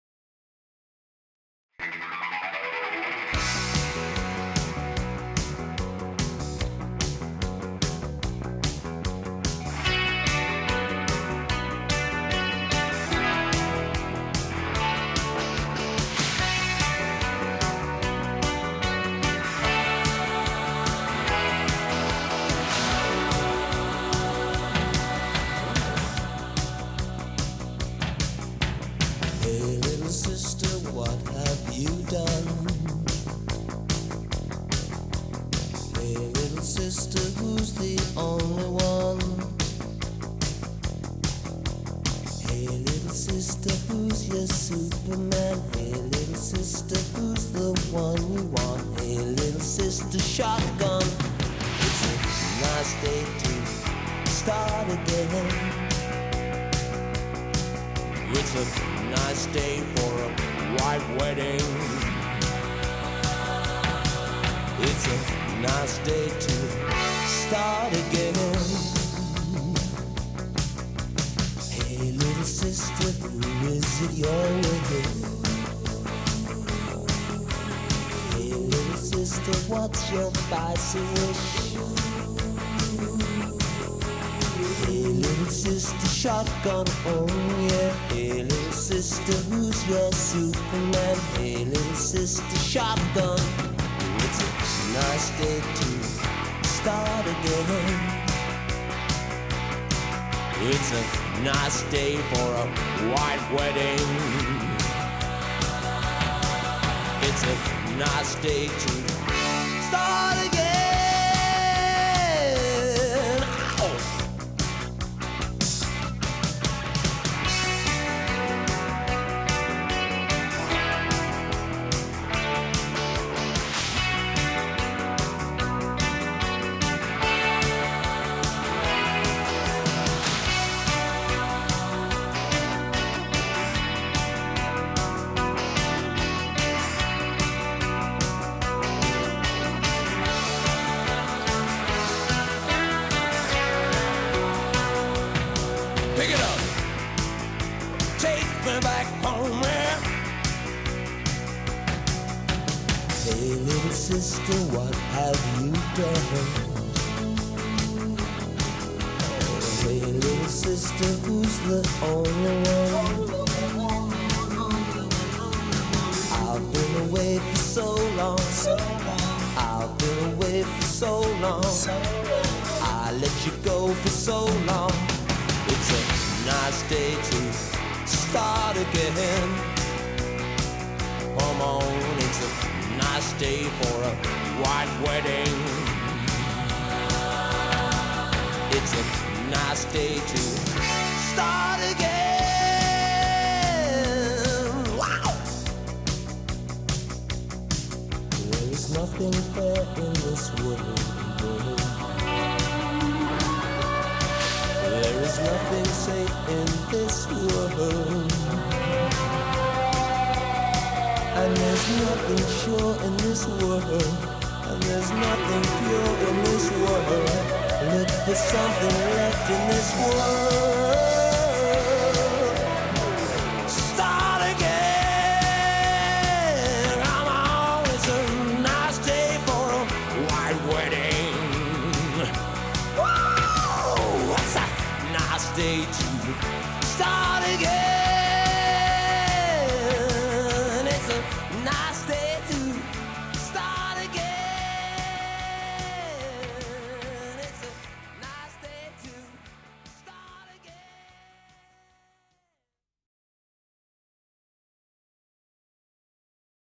White Wedding Nightmare of 2020
Ever since we got married, life has been a downward spiral of shit. If we survive this, we can survive anything. Maybe this will help.